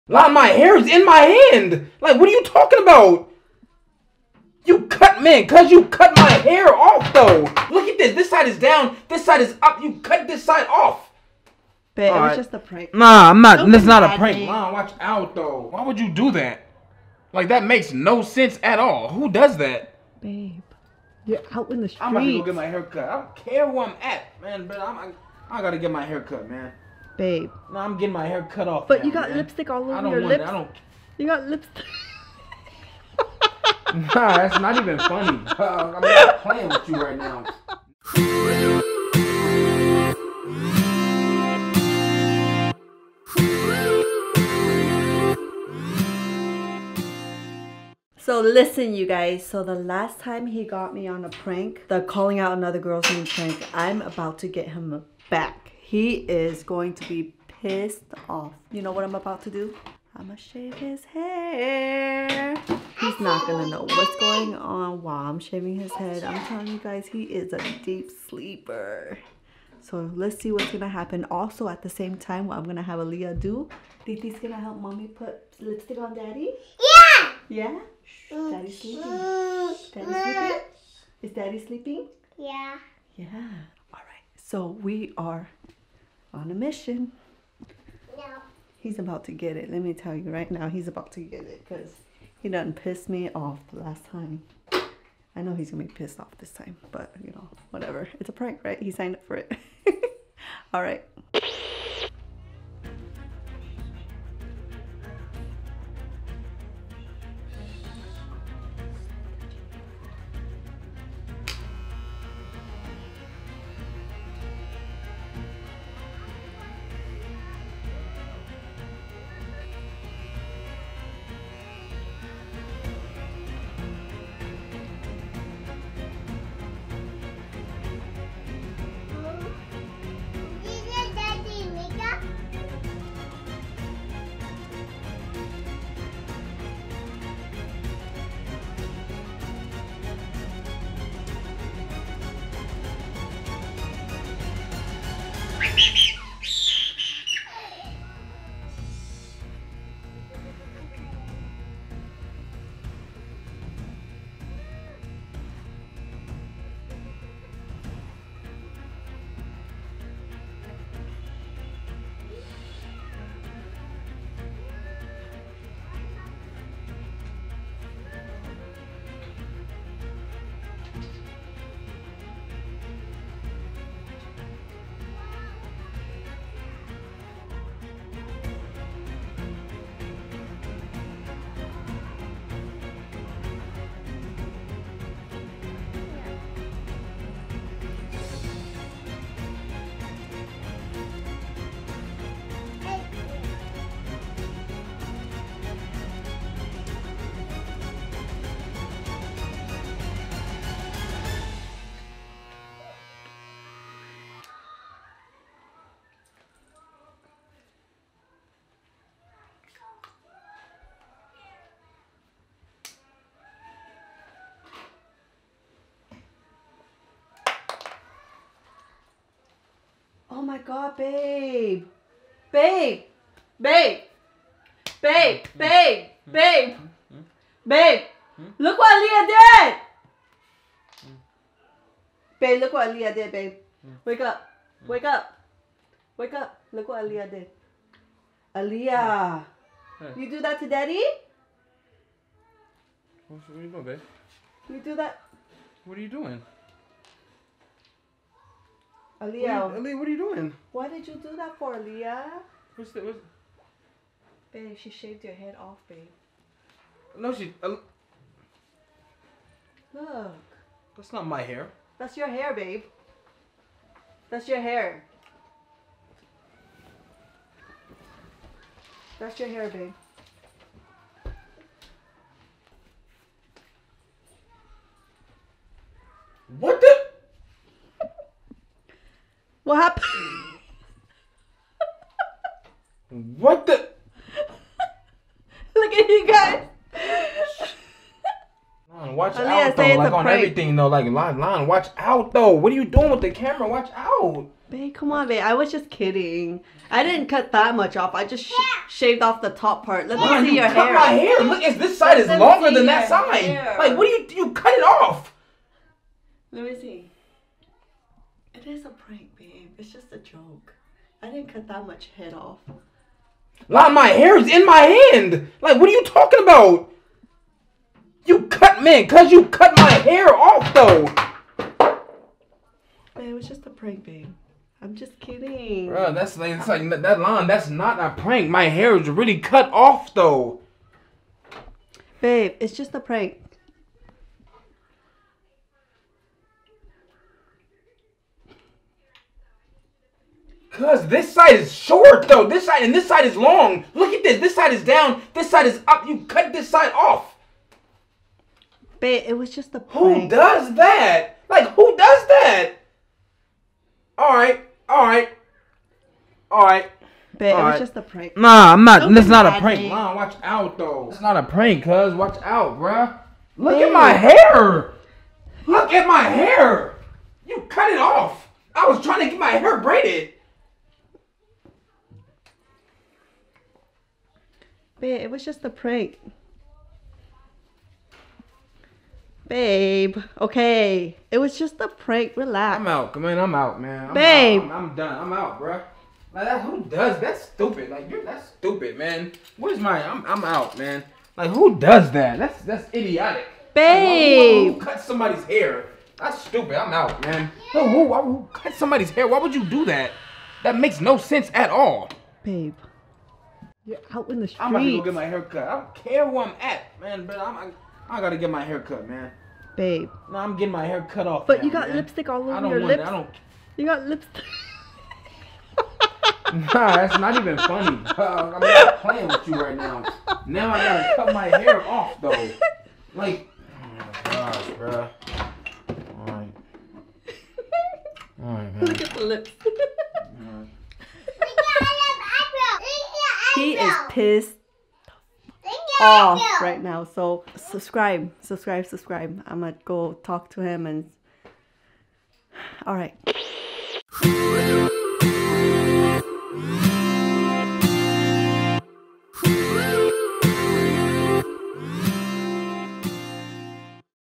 Cutting My Husbands Hair While he Sleeps!!!!!PRANK
If you watched the last prank, you already know that Steph called out another girls name in his sleep and that is a no no. Now it's payback time.

Stephone & Lan THE BLASIAN FAMILY